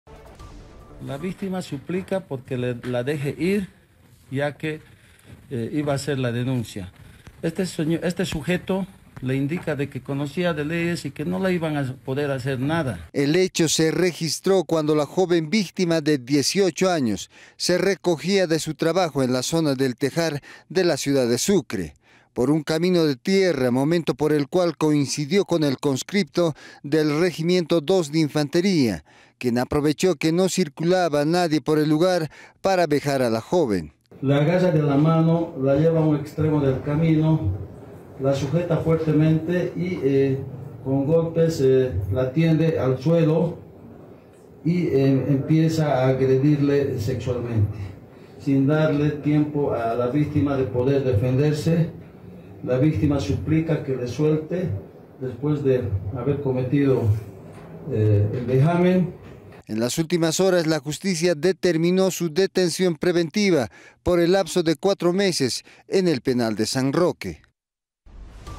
Detención preventiva para un conscripto acusado de violación a una joven
Sucre | Detención preventiva para un conscripto acusado de violación a una joven de 18 años.